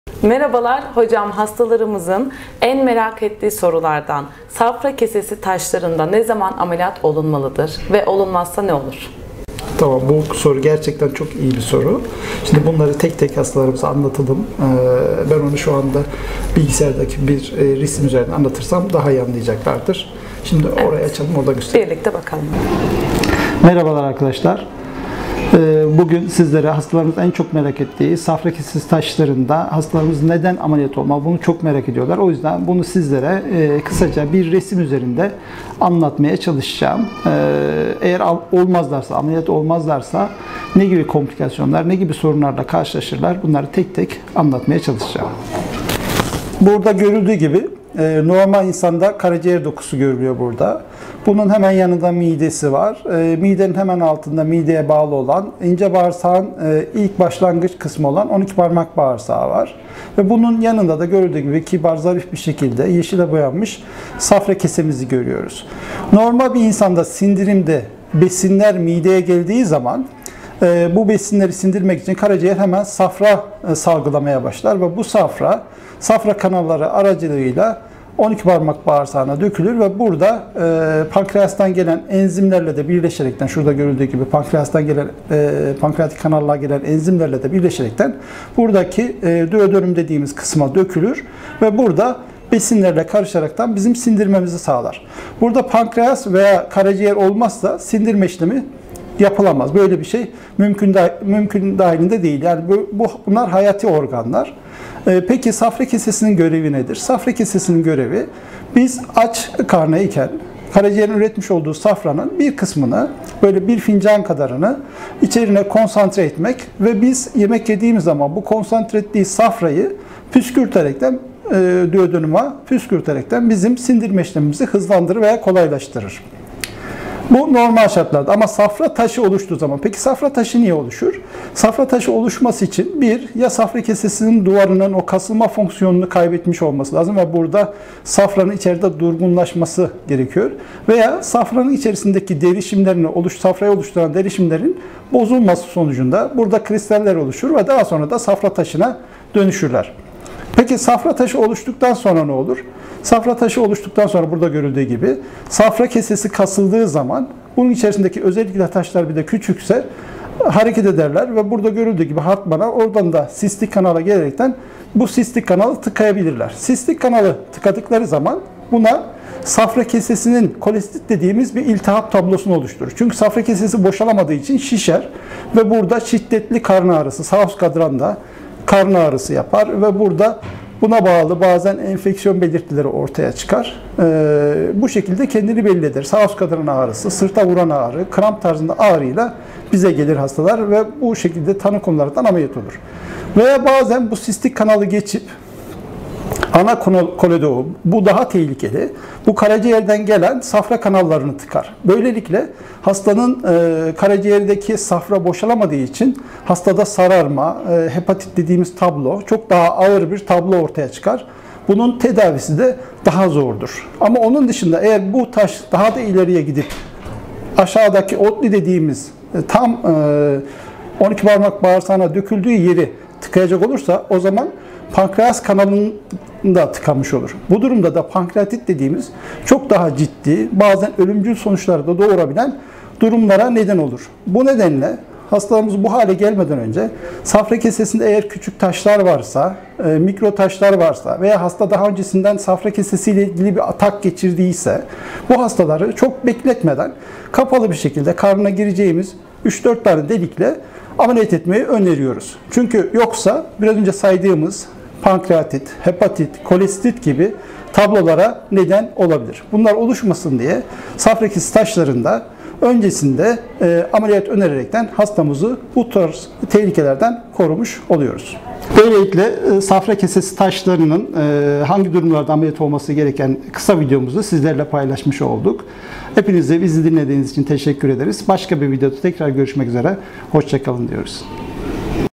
Safra kesesi taşlarında neden ameliyat olmalı
Safra  kesesi taşlarında neden ameliyat olmalı? olmazlar ise ne gibi sorunlara neden olmaktadır. Hangi durumlarda ameliyat olmalıdır?